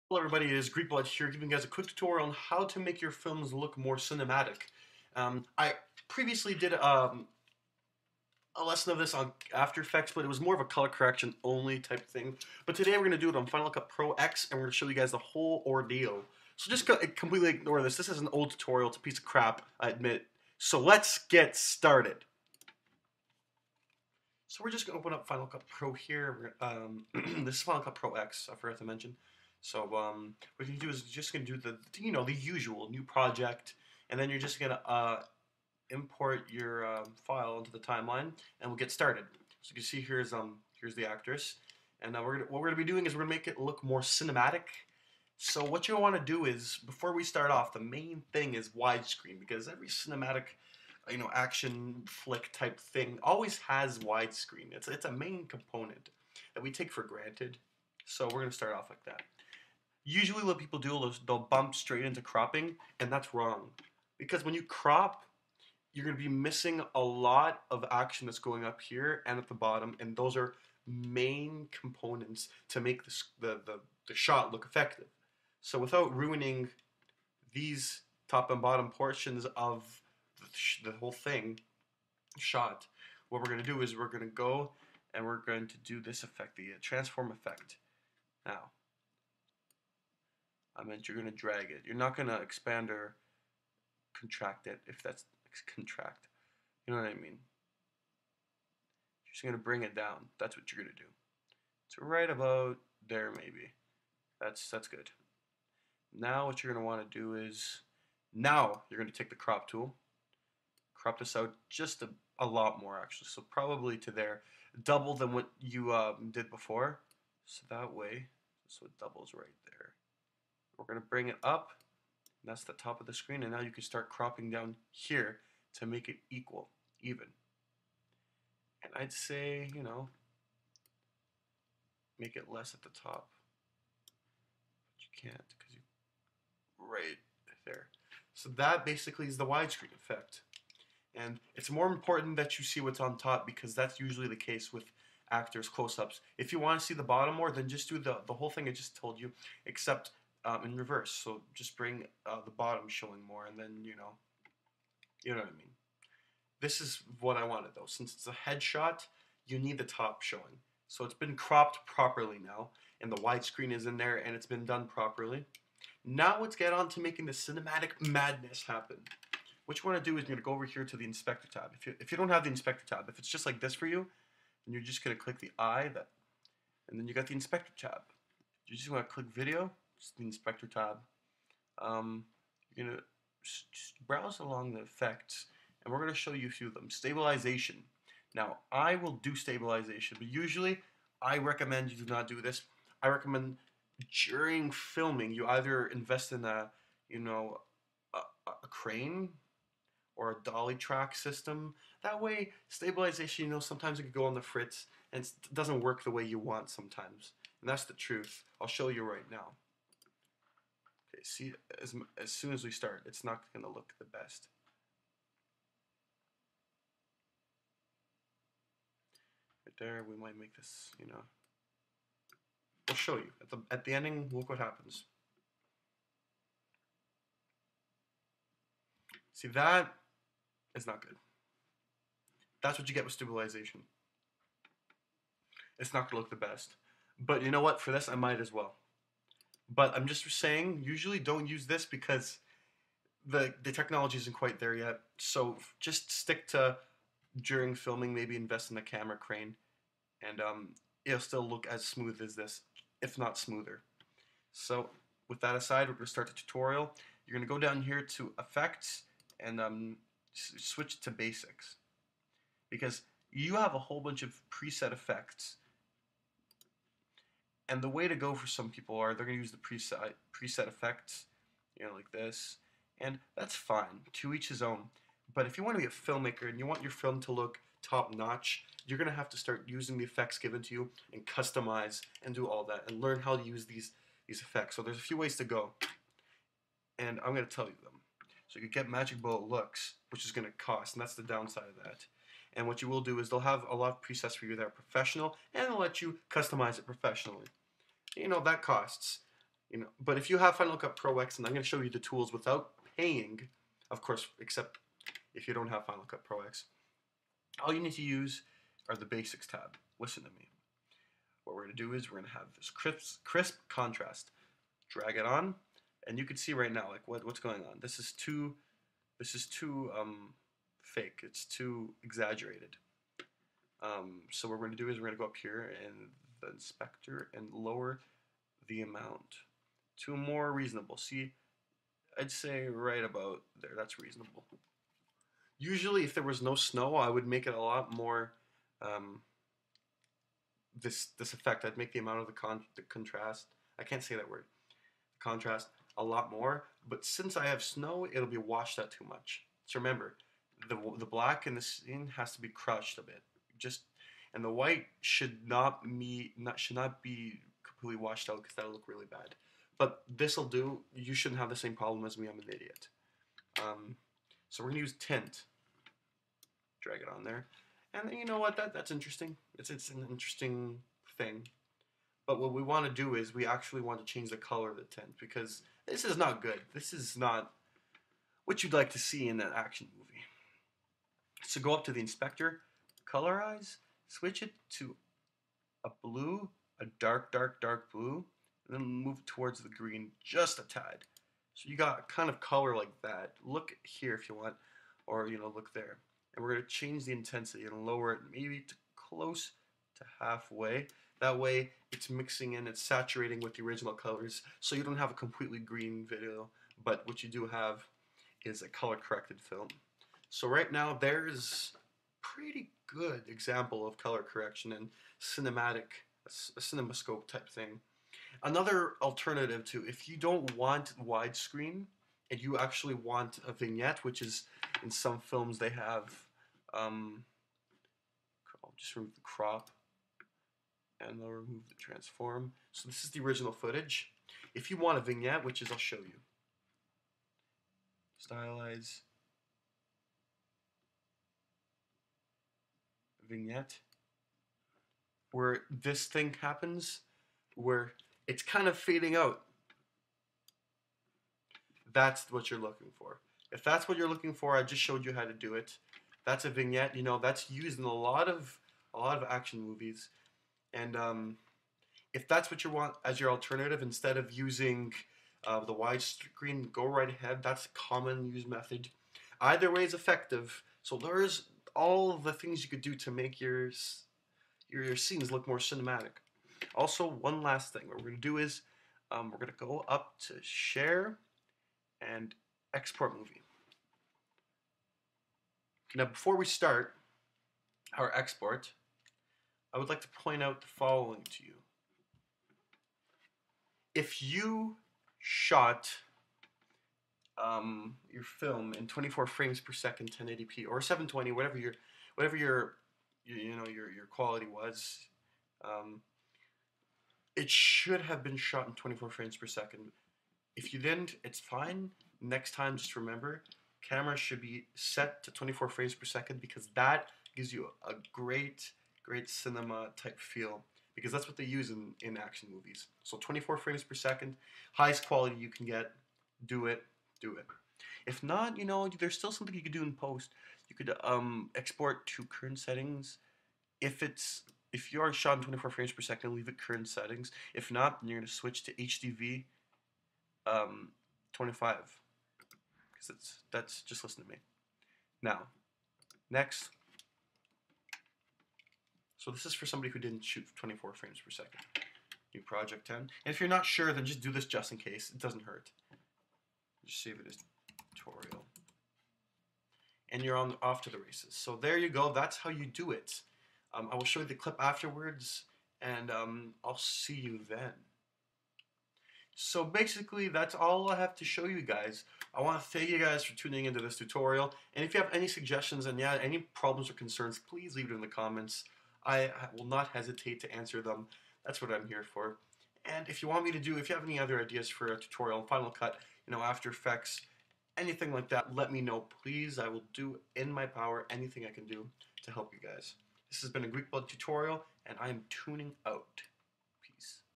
How To: Making Films More Cinematic On Final Cut Pro X
How To Make Films More Cinematic On Final Cut Pro X.mp4